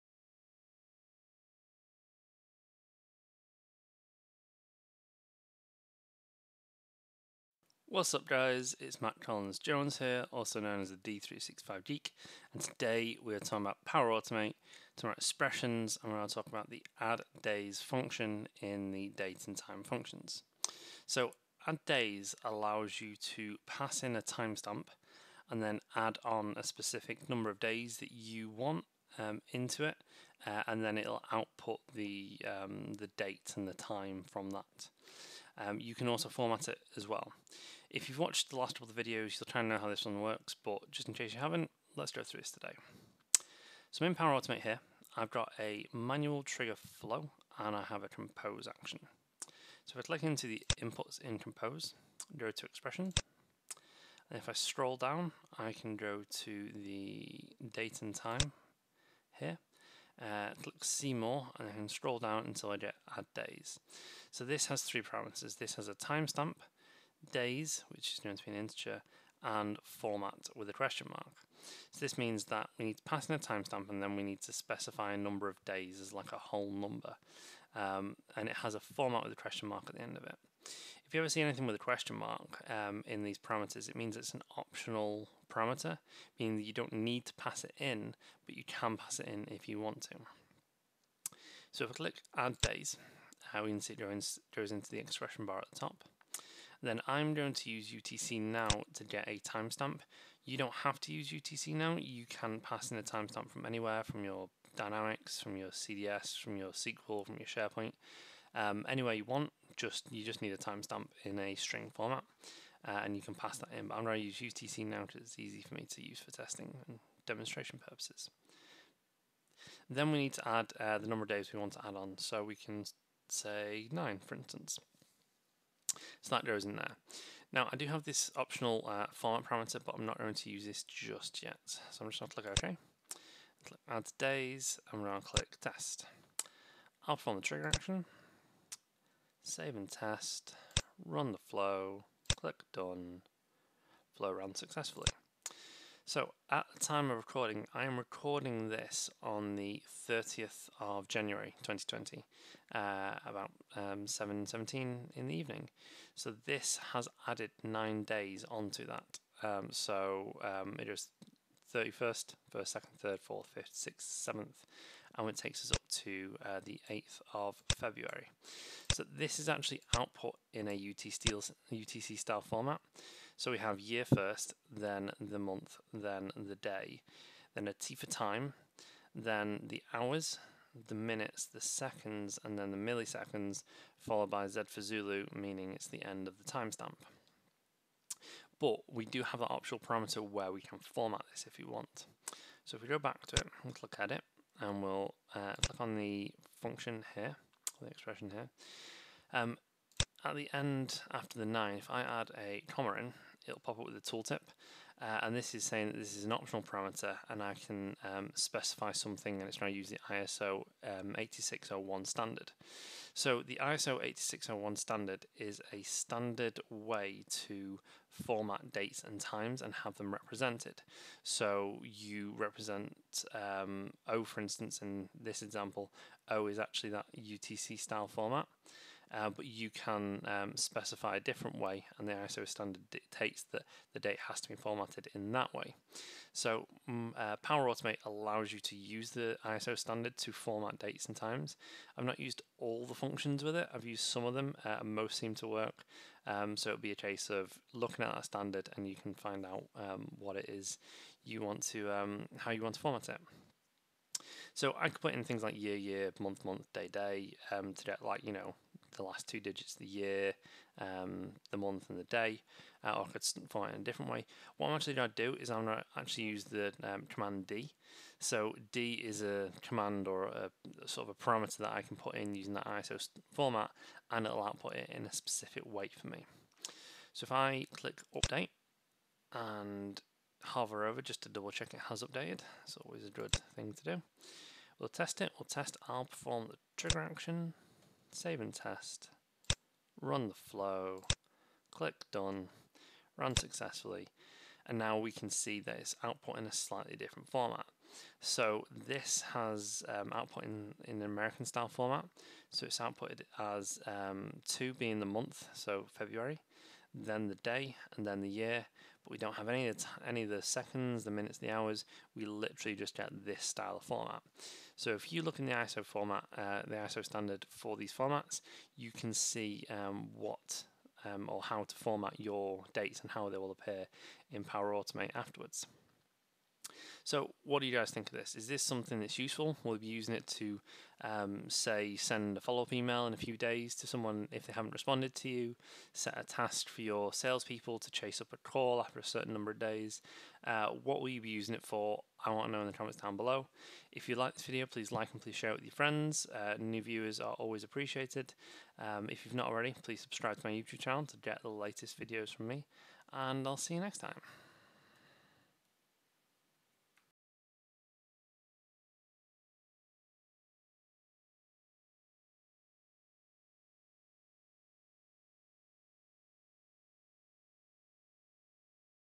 Power Automate Expressions How To: addDays
In this video, I go through the addDays Expression in Power Automate. Power Automate is a powerful automation and integration tool with over 280+ data connectors.